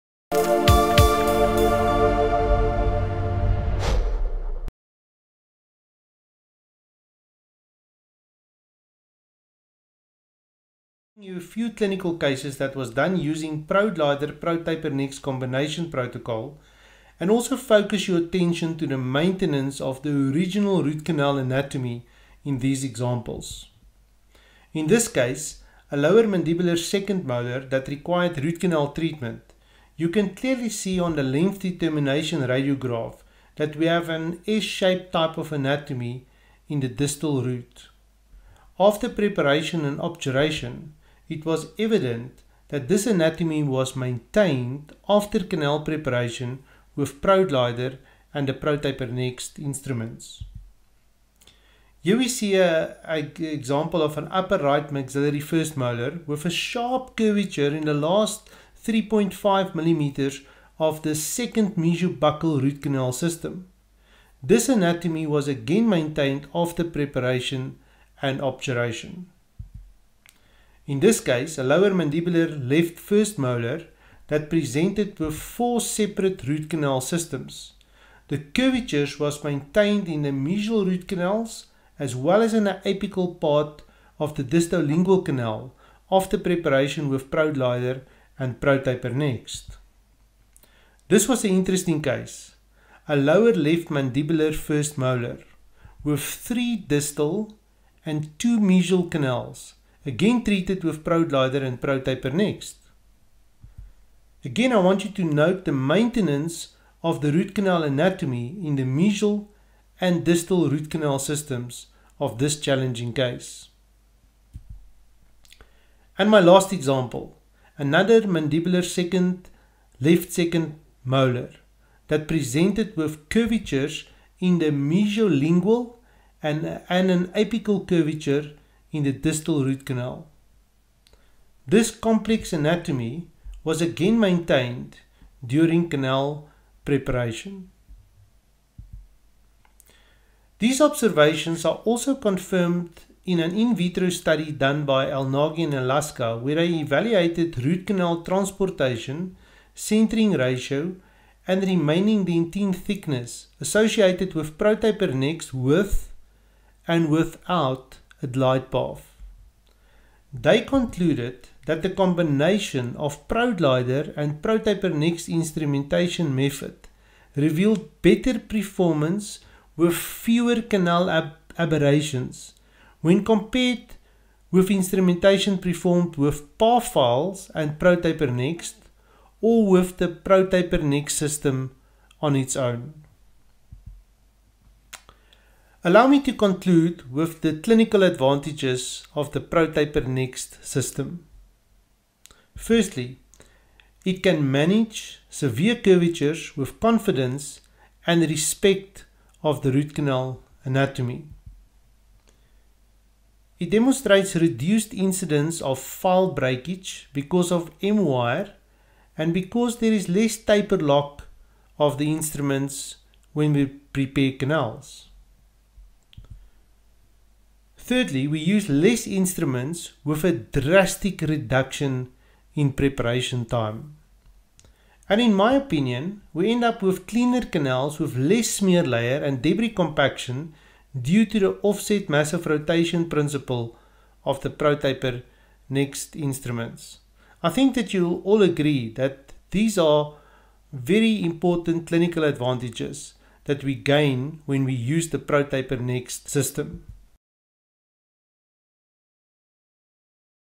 ProTaper Next® Clinical Studies with Dr. Peet van der Vyver | Dentsply Sirona
Dr. Peet van der Vyver shows clinical case studies that used ProGlider® and ProTaper Next® and the clinical advantages